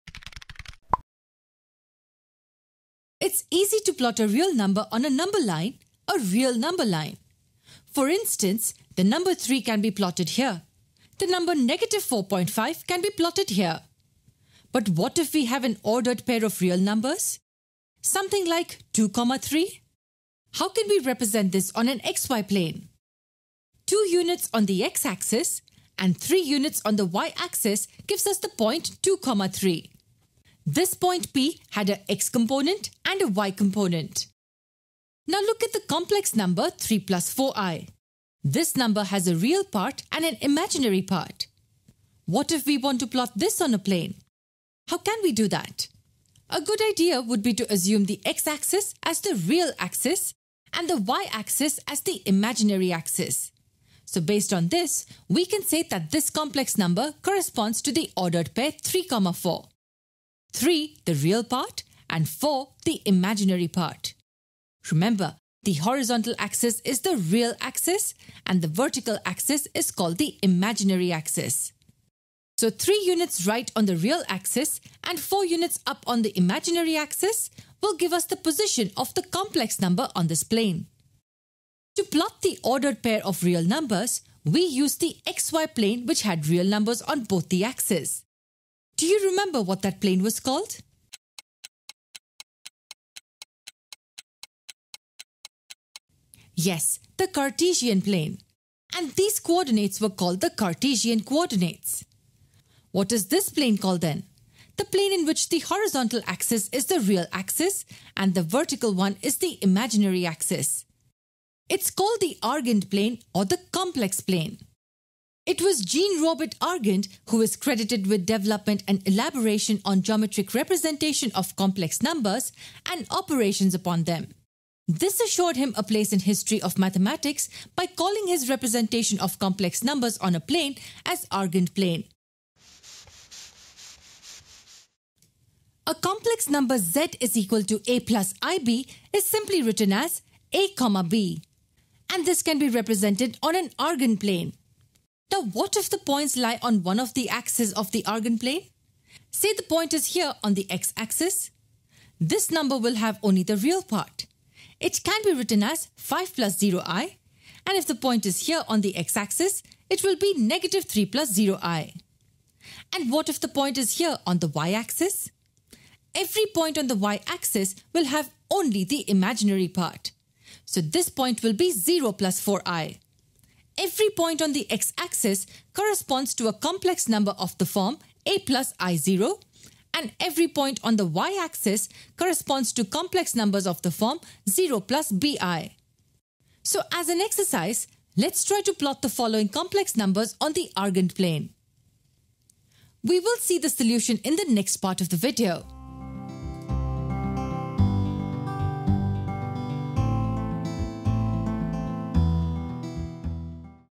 What is Argand Plane? Part 1 | Complex Numbers | Don't Memorise
📅🆓NEET Rank & College Predictor 2024 What is the Argand Plane? Watch this video to know more

To access all videos related to Complex Numbers, enrol in our full course now